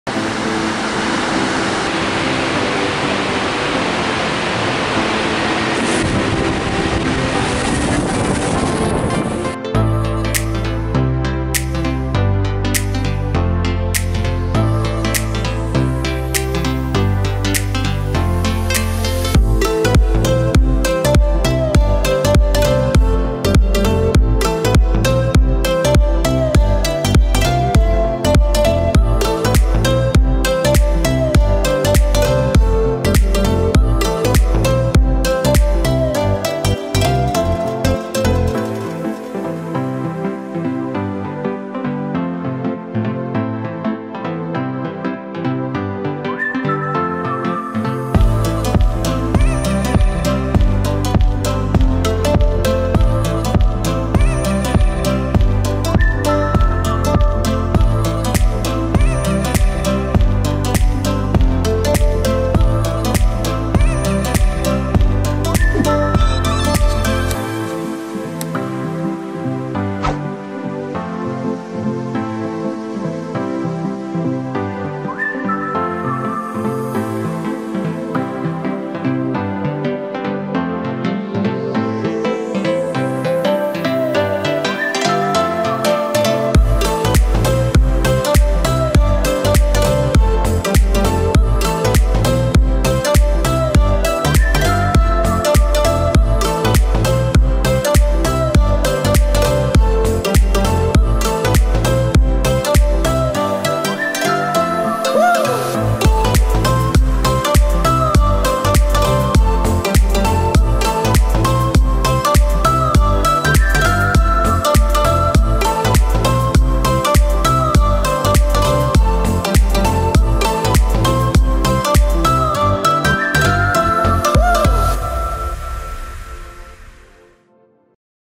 Wodospad Langfossen. OKIEM DRONA
Kilka ujęć piątego co do wielkości wodospadu w Norwegii. Spadek wynosi 612 metrów